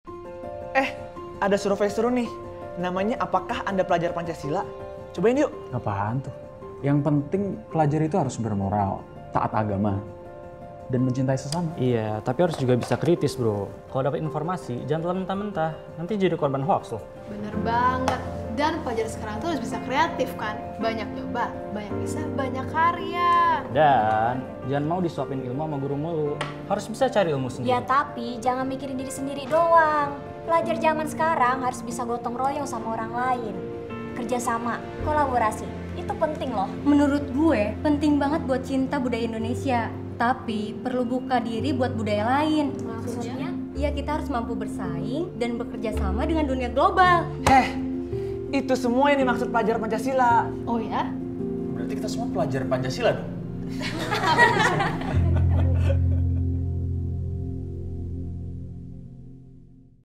Profil Pelajar Pancasila
SahabatKarakter, tahukah kamu ada enam indikator Pelajar Pancasila. Keenam indikator tersebut adalah beriman, bertakwa kepada Tuhan YME dan berakhlak mulia, mandiri, bernalar kritis, greatif, bergotong royong dan berkebinekaan global.

Profil Pelajar Pancasila inilah yang akan mampu menyiapkan generasi muda menghadapi tantangan global dan revolusi industri 4.0. Bukan hal yang sulit untuk menunjukan kecintaan Pancasila di masa kini, yaitu dengan praktik kehidupan sehari-hari. Nah apakah SahabatKarakter seorang Pelajar Pancasila? Bagikan di kolom komentar ya!